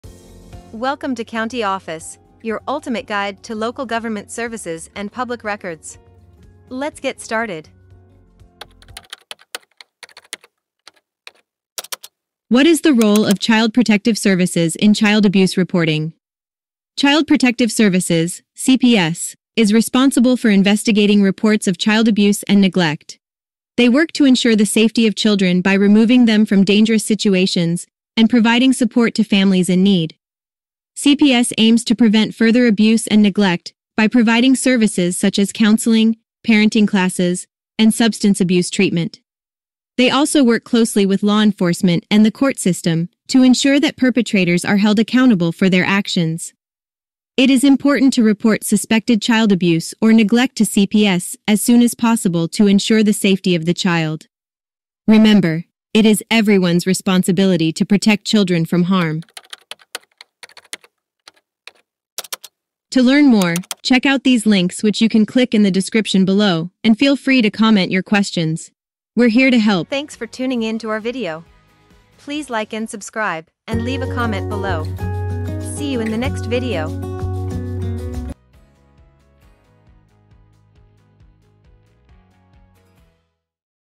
What Is The Role Of Child Protective Services In Child Abuse Reporting? - CountyOffice.org
What Is The Role Of Child Protective Services In Child Abuse Reporting? Child abuse is a serious issue that affects countless children every year. That's why Child Protective Services (CPS) plays such a vital role in our society. In this video, we'll explore the important work that CPS does to investigate reports of child abuse and neglect.

We'll discuss how CPS works to ensure the safety of children by removing them from dangerous situations and providing support to families in need. We'll also explain the various services that CPS offers to prevent further abuse and neglect, including counseling, parenting classes, and substance abuse treatment.

Additionally, we'll take a look at how CPS works with law enforcement and the court system to hold perpetrators accountable for their actions. We'll also stress the importance of reporting suspected child abuse or neglect to CPS as soon as possible to ensure the safety of the child.

If you're concerned about child abuse or want to learn more about CPS, this video is a must-watch.